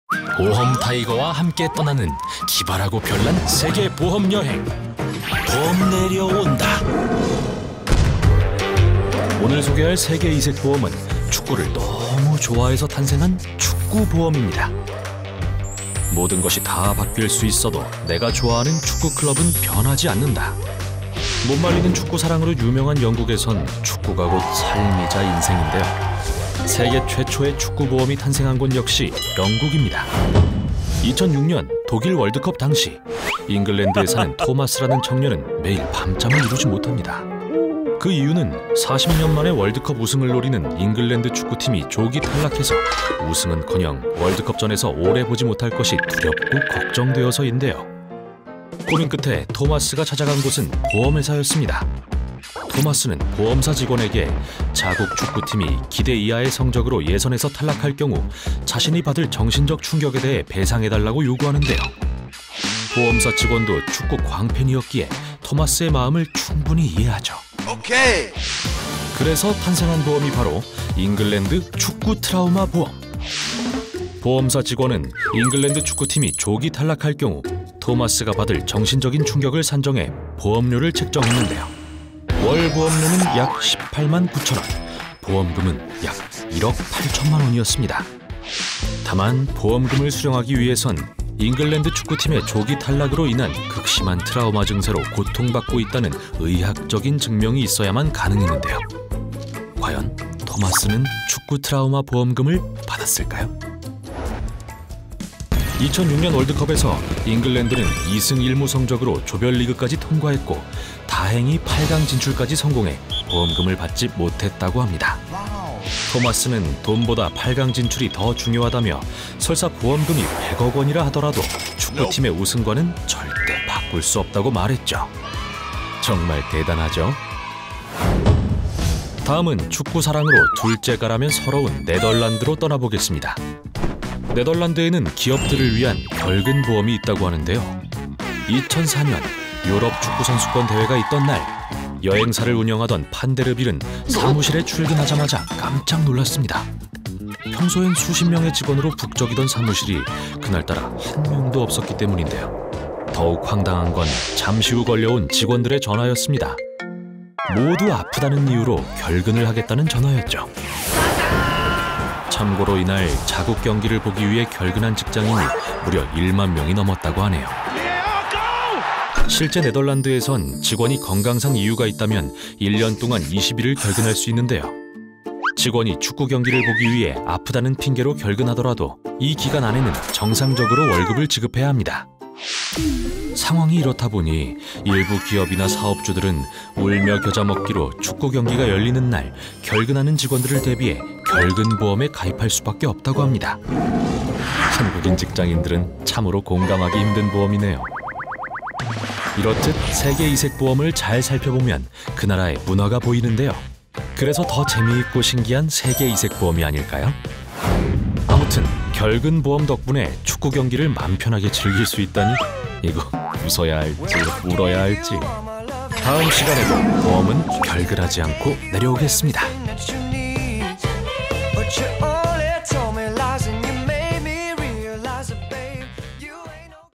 월드컵 예선에 떨어질 경우를 대비한 보험이 있다?! ⚽ 신비한 보험의 세계 [보험 내려온다]
전세계적으로 뜨거운 사랑을 받는 인기 스포츠, 축구! ⚽
박진감 넘치는 경기와 매력 속으로~
심지어 영국과 네덜란드에는 유별난 축구 사랑으로 생겨난
이색 보험까지 존재한다고 하는데...?
월드컵 예선에 진출하지 못할 경우
보험금으로 보상받을 수 있는 축구트라우마 보험부터
축구 경기가 있는 날마다 결근하는 직원들을 위해
사장님이 가입해야하는 결근보험까지 😱
축구를 너무나 사랑한 나머지 생기게 된,
웃지 못할 축구보험, 함께 알아보시죠!